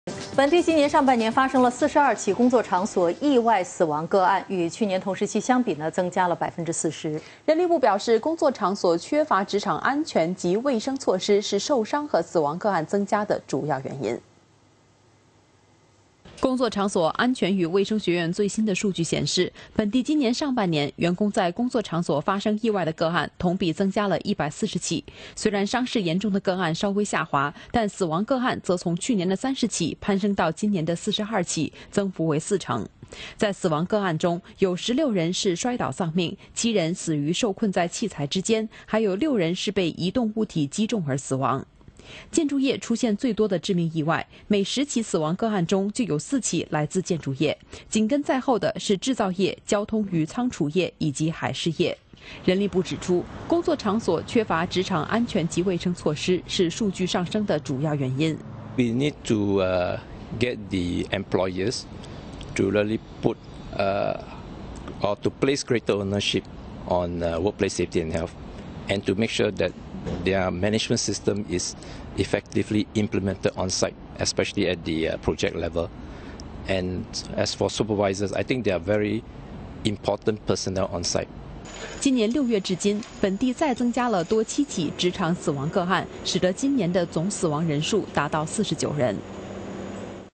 工作场所缺乏安全及卫生措施 是伤亡增加的主因
本地今年上半年发生了42起工作场所意外死亡个案，与去年同时期相比，增加了40%。人力部表示，工作场所缺乏职场安全及卫生措施，是受伤和死亡个案增加的主要原因。